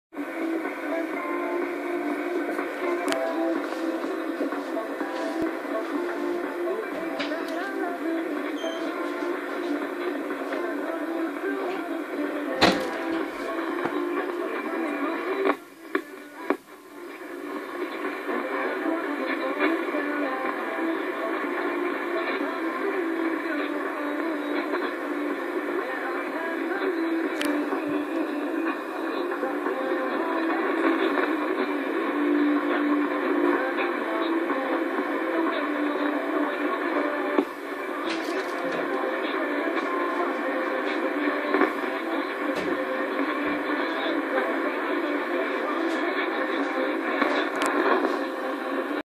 Hit Radio 828 Khz, emisora que emite desde Barcelona, desde las instalsciones de la Ex-Radio Terrasa
Escuchada el día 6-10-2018 en Cartagena-Murcia (España). Hora de la escucha 1:15 UTC